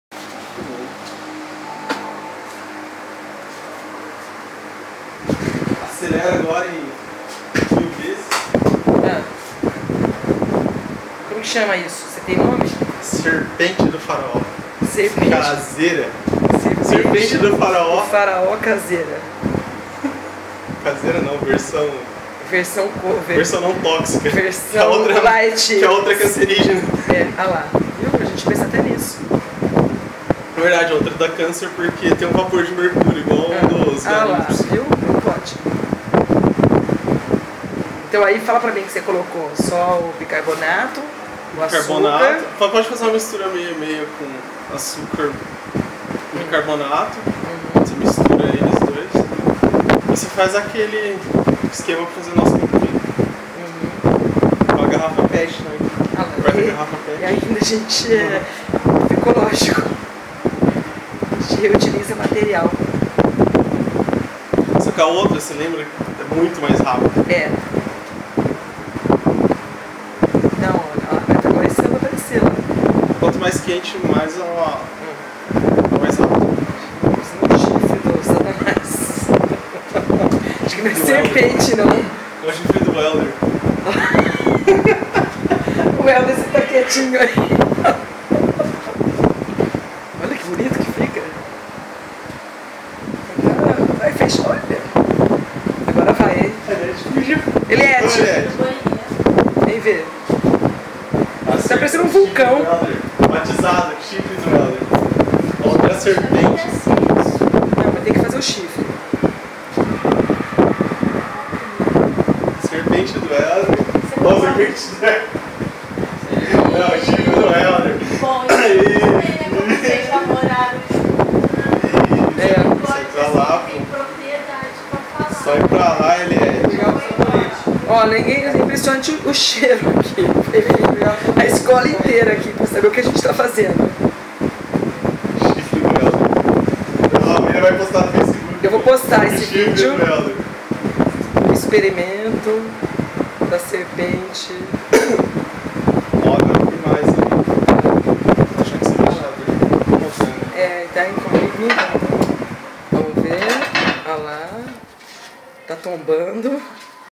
Serpente do Faraó ecológica
Experimento feito pelo Prof Wellington Pina com assistência da prof Mirian Martinez.Não filmei o melhor, pois esgotou  a memória do celular... Repetiremos , com certeza!!!